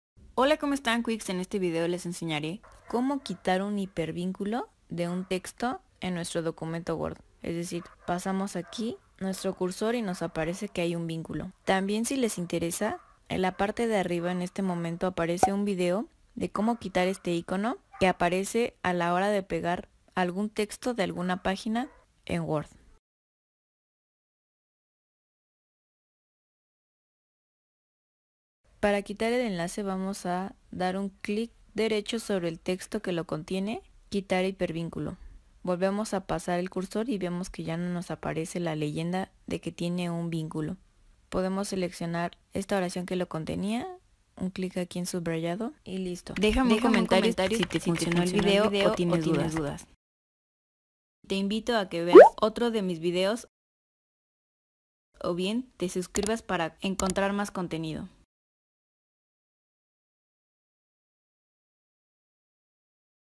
Cómo QUITAR un HIPERVÍNCULO de Word ❌
✔️Tutorial de Cómo Eliminar un Hipervínculo (enlace) de un Texto que fue Pegado en Word.
🔻Mira aquí abajo 👀

¿Necesitas un tutorial? sugierelo en los comentarios 👇🏻

Agradeceré mucho tu comentario y like 👍🏻